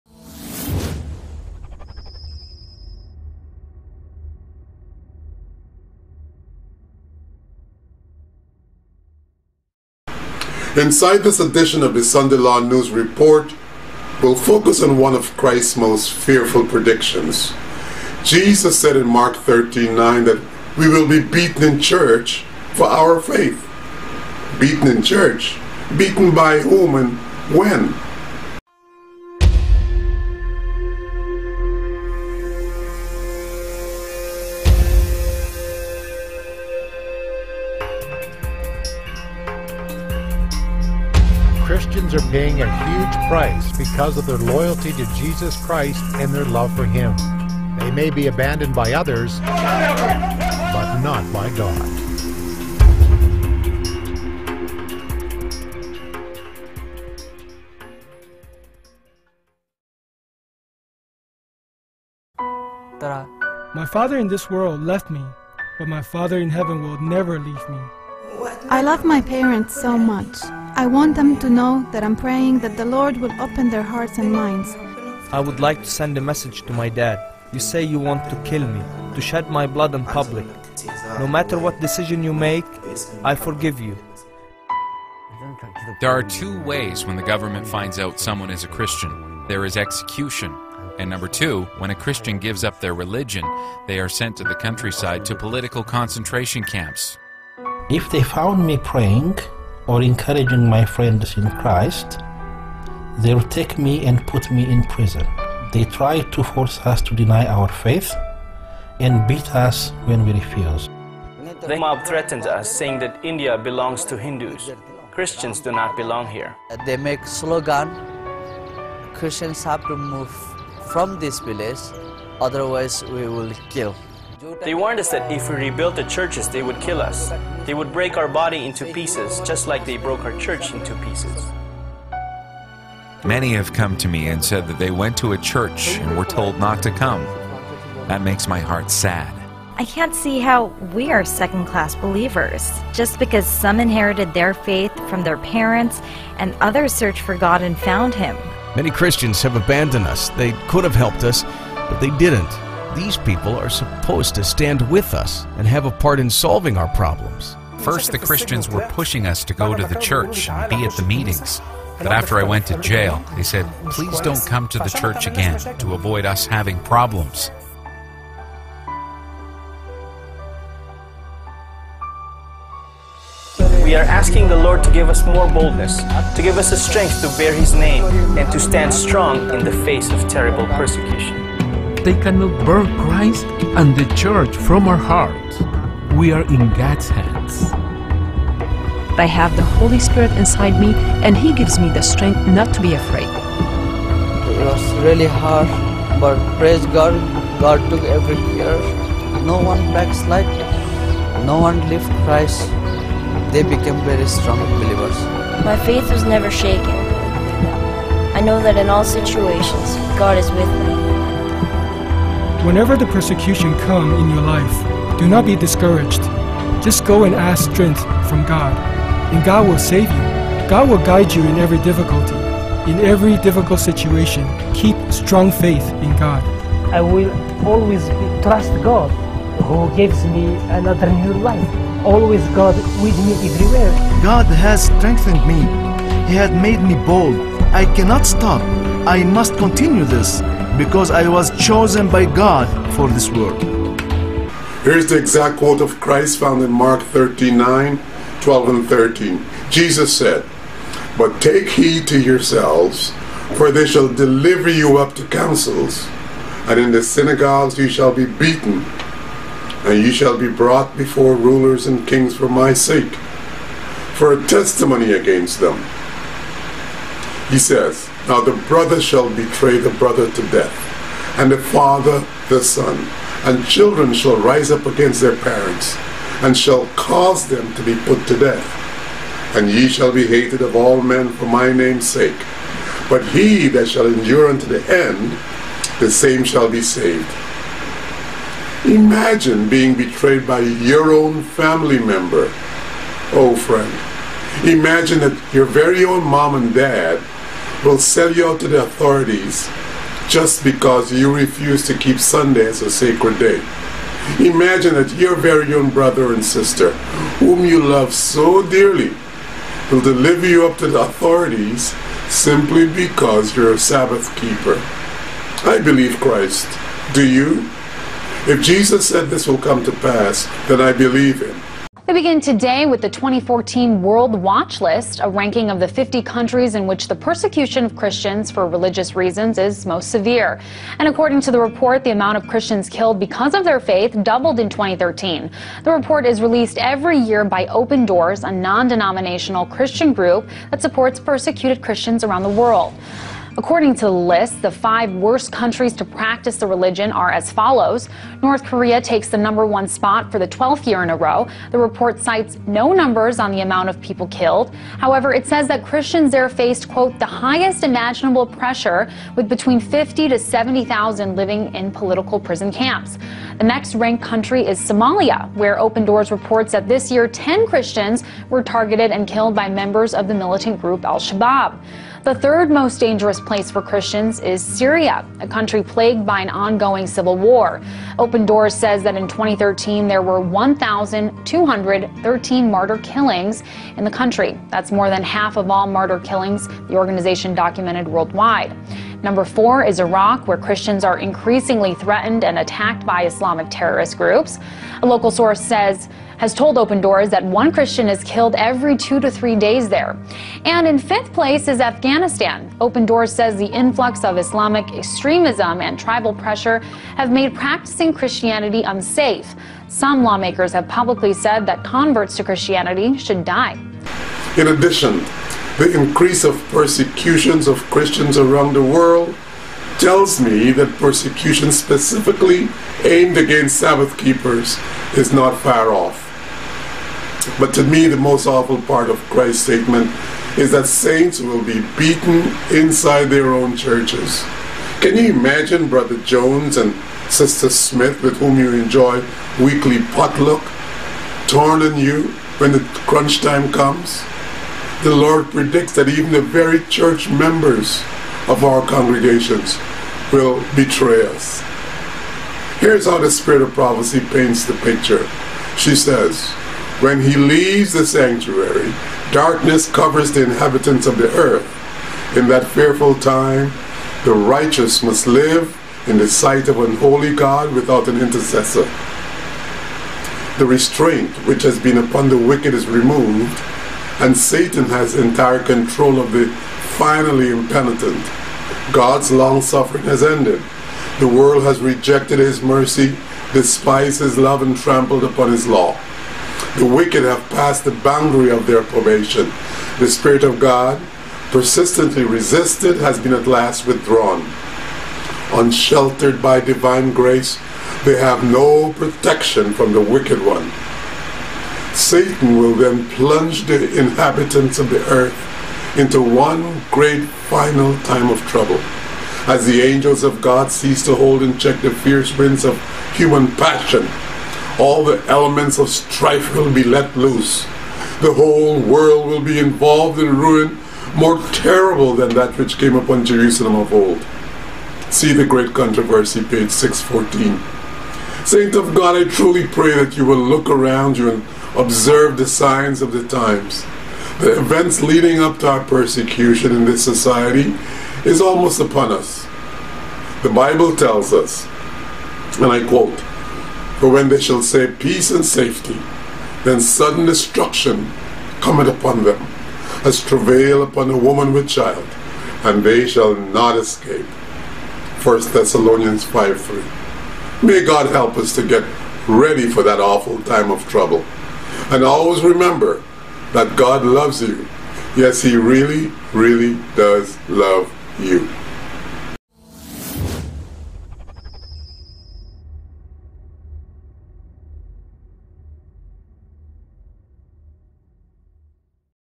Sunday Law News Report - Beaten Inside Church
- Imagine being betrayed by your own family member!
Imagine that your very own mom and dad will sell you out to the authorities, just because you refuse to keep Sunday as a sacred day. Imagine that your very own brother and sister whom you love so dearly, will deliver you up to the authorities simply because you're a Sabbath keeper.

Our discussion inside this edition of The Sunday Law News Report focuses on being beaten in church.

Can imagine that Bro. Jones and Sis. Smith with whom you enjoy weekly potluck lunches will turn on you when crunch time comes?

The Lord predicts that even the very members of our church congregations will betray us.

"When He leaves the sanctuary, darkness covers the inhabitants of the earth. In that fearful time the righteous must live in the sight of a holy God without an intercessor.

The restraint which has been upon the wicked is removed, and Satan has entire control of the finally impenitent. God's long-suffering has ended. The world has rejected His mercy, despised His love, and trampled upon His law.

The wicked have passed the boundary of their probation; the Spirit of God, persistently resisted, has been at last withdrawn. Unsheltered by divine grace, they have no protection from the wicked one.

Satan will then plunge the inhabitants of the earth into one great, final trouble. As the angels of God cease to hold in check the fierce winds of human passion, all the elements of strife will be let loose.

The whole world will be involved in ruin more terrible than that which came upon Jerusalem of old." The Great Controversy, Page 614.